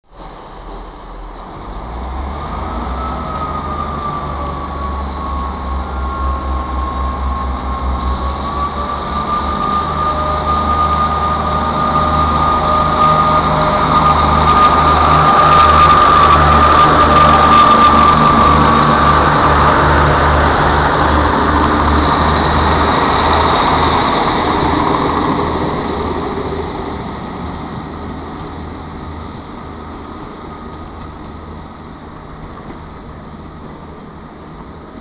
158960 1F26 04/03/12 Westbury
158960 is seen leavnig Westbury on the 04/03/12 with the 1F26 15:08 Portsmouth Harbour-Brisrol Parkway FGW service. It left Westbury 17 minutes late.